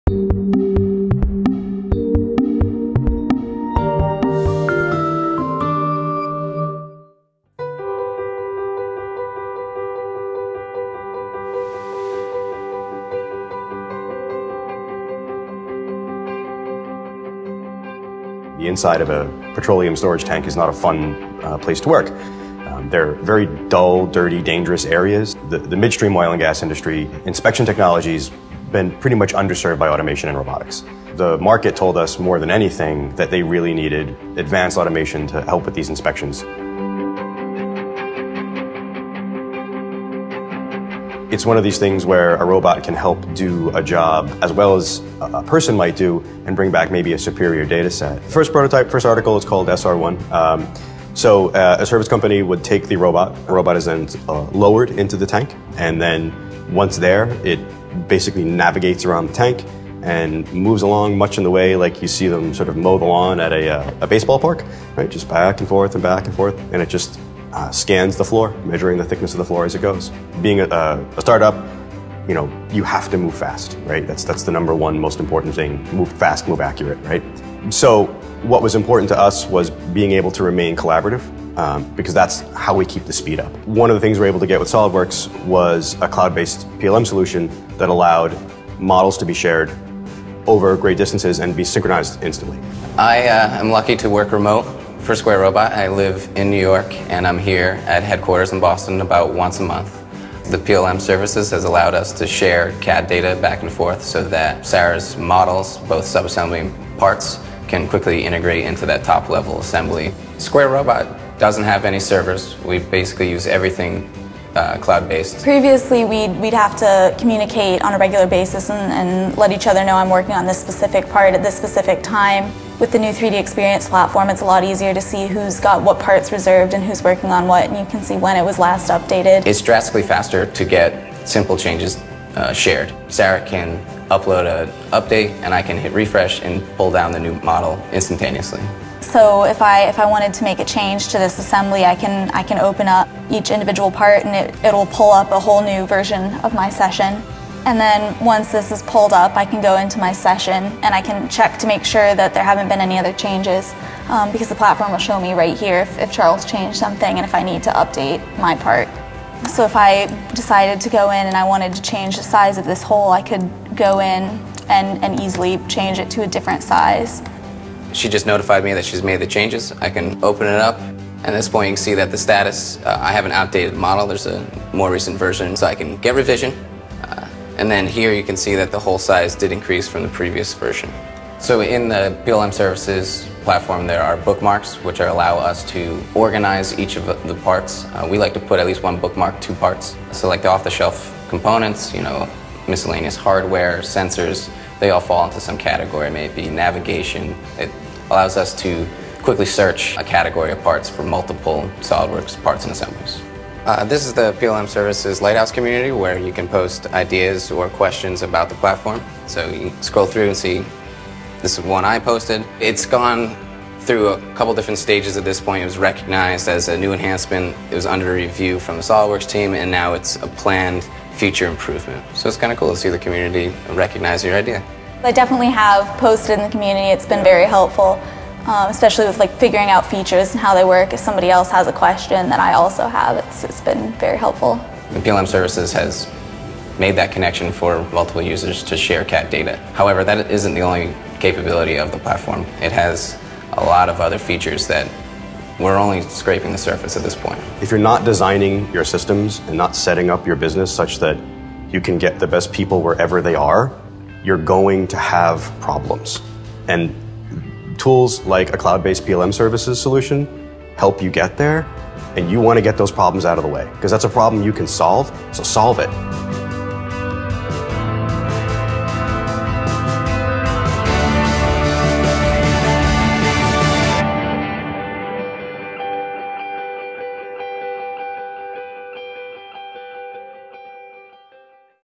Square Robot - Autonomous Inspection in Harsh Environments - SOLIDWORKS
Square Robot - Autonomous Inspection in Harsh Environments - SOLIDWORKS. Square Robot is revolutionizing the inspection of oil tanks with an autonomous robot that can perform assessments without the disruption. As a startup, they need to move quickly and focus on getting their product to market, rather than on deploying software. Watch how 3DEXPERIENCE PLM Collaboration Services allowed them to improve design team collaboration, shorten time to market and establish data fidelity and digital continuity.